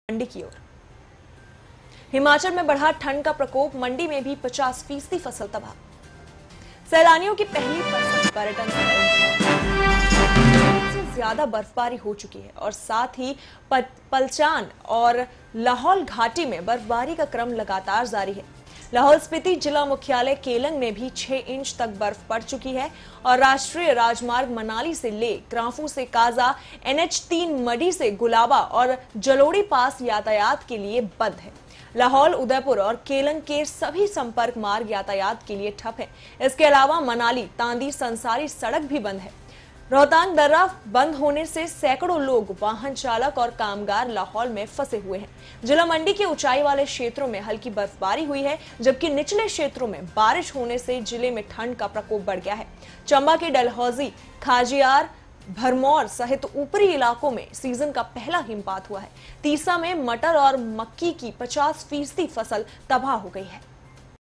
MANDI: हिमाचल में बढ़ा ठंड का प्रकोप, 50 फीसद फसल तबाह
सैलानियों की पहली पसंद पर्यटनस्थल रोहतांग दर्रे में तीन फीट से अधिक बर्फबारी हो चुकी है। साथ ही पलचान और लाहुल घाटी में बर्फबारी का क्रम जारी है। लाहुल स्पीति जिला मुख्यालय केलंग में भी छह इंच तक बर्फ पड़ चुकी है। राष्ट्रीय राजमार्ग मनाली से लेह, ग्रांफू से काजा, एनएच-तीन मढ़ी से गुलाबा और जलोड़ी पास यातायात के लिए बंद हैं।